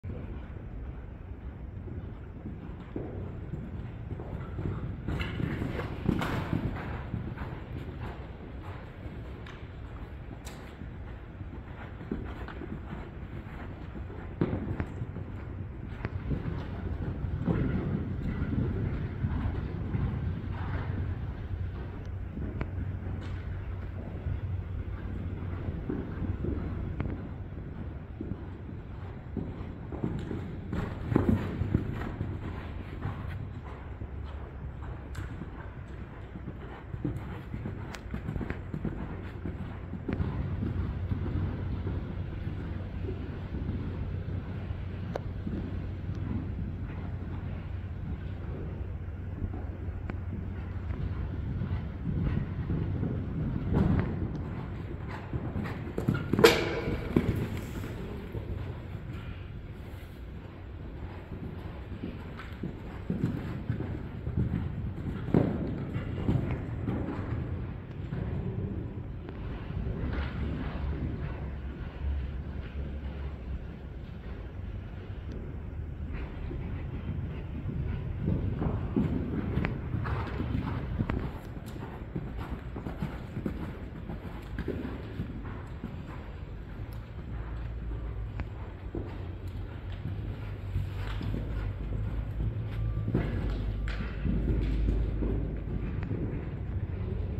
Zachert (Elvaro x Zacharov) 2023/11 - training
Zachert             Gelding
Year                   2018
Breed                 PZHK
Pedigree           Elvaro x Zacharov x Larino
Height               170 cm / 16.3 hh
Discipline          Show Jumping
x-rays                 yes
Location            Poland

Zachert was recently started and is acting beautifully under the saddle. He is coming from very well minded lines where both his sire Elvaro (Calvaro x Heartbreaker) and his dam Farina (by Zacharov) are proven to give great attitude. He has balanced canter and scopy jump which makes him an interesting project for the future. On the arena’s we can watch Zachert’s older sister Zabawa (by Biscayo) who was showing on 130 cm level as a 6 yo together with a junior girl.